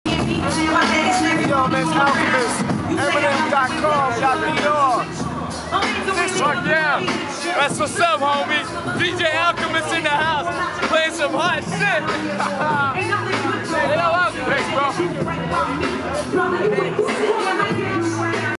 DJ do Eminem, The Alchemist mandando um salve ao EminemBrasil
DJ Alchemist sending a shot out to EminemBrasil at Clash Club in São Paulo, Brazil.